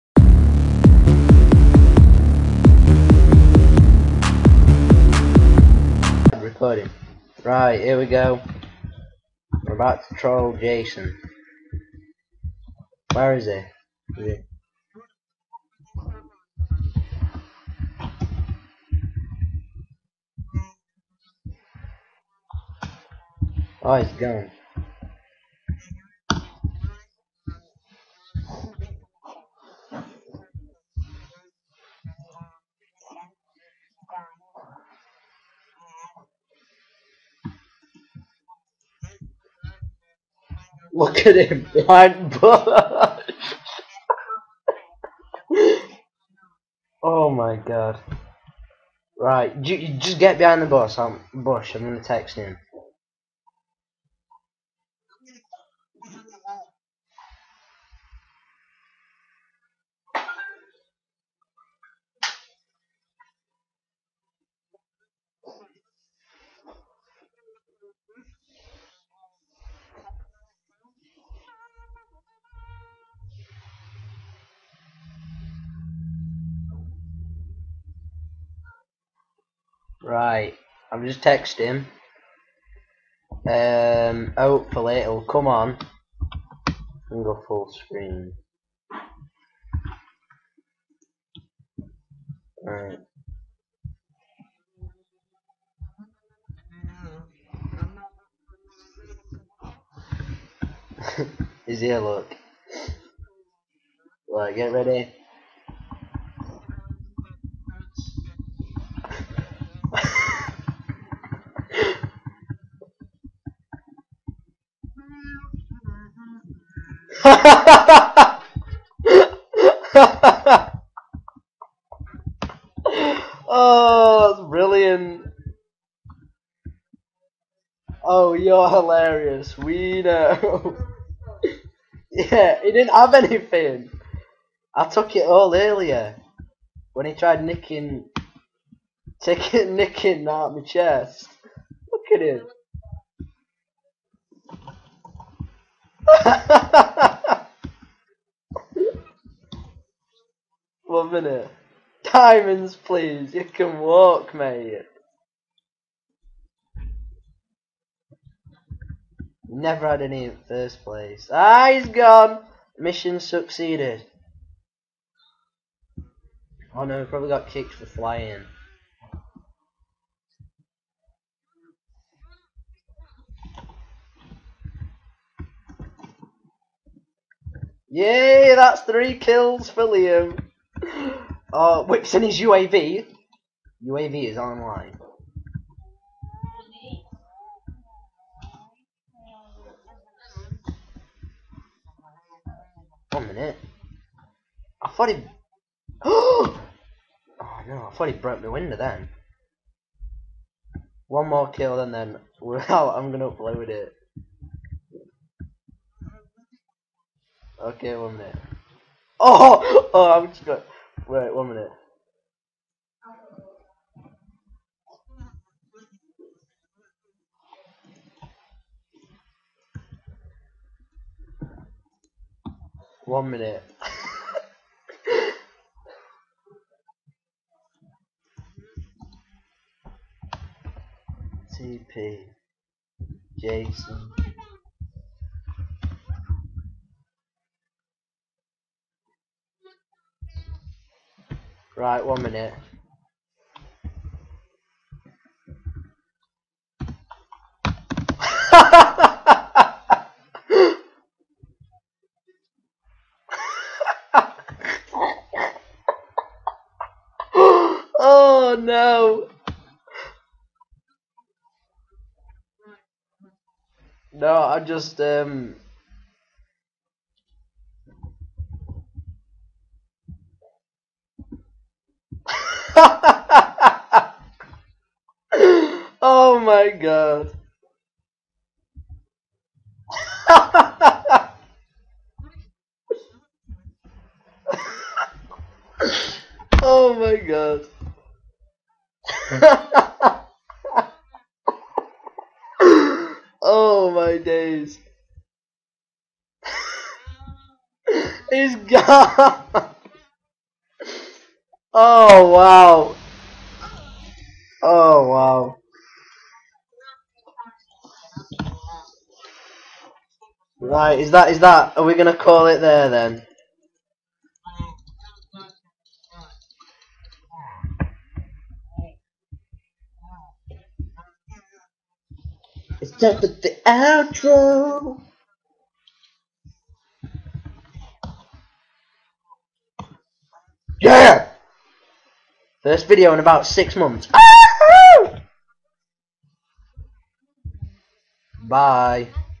Ultimate Troll
Jason had been taking from my chest so it was payback time! Liam and Lewis tag team Jason in a trolling fest!!